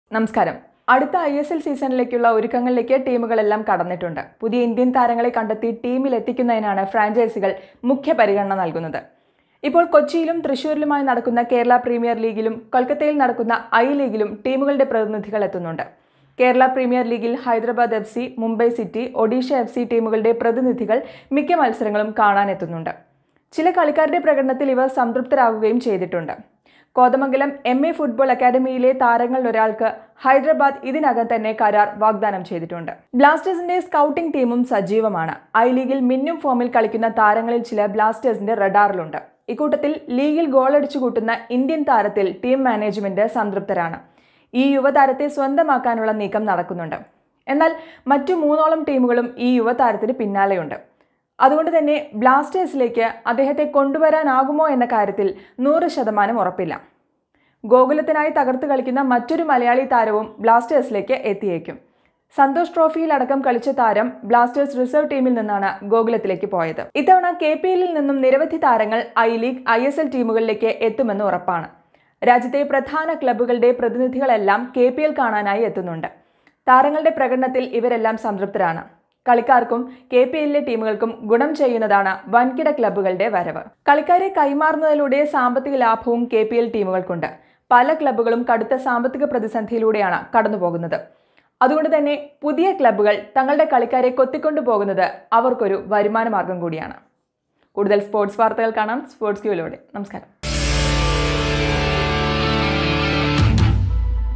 ഐലീഗ്‌ സൂപ്പര്‍താരത്തിനായി ബ്ലാസ്റ്റേഴ്‌സ്‌, കെപിഎല്ലിലേക്കും നോട്ടം! | isl clubs in kpl
ഇത്തവണത്തെ കെപിഎല്ലില്‍ നിന്നും നിരവധി താരങ്ങള്‍ ഐലീഗ്‌, ഐഎസ്‌എല്‍ ടീമുകളിലേക്ക്‌ എത്തുമെന്ന്‌ ഉറപ്പാണ്‌. രാജ്യത്തെ പ്രധാന ക്ലബുകളുടെ പ്രതിനിധികളെല്ലാം കെപിഎല്‍ കാണാനായി എത്തുന്നുണ്ട്‌. താരങ്ങളുടെ പ്രകടനത്തില്‍ ഇവരെല്ലാം സംതൃപ്‌തരാണ്‌. കളിക്കാര്‍ക്കും കെപിഎല്ലിലെ ടീമുകള്‍ക്കും ഗുണം ചെയ്യുന്നതാണ്‌ വന്‍കിട ക്ലബുകളുടെ വരവ്‌. കളിക്കാരെ കൈമാറുന്നതിലൂടെ സാമ്പത്തികലാഭവും കെപിഎല്‍ ടീമുകള്‍ക്കുണ്ട്‌.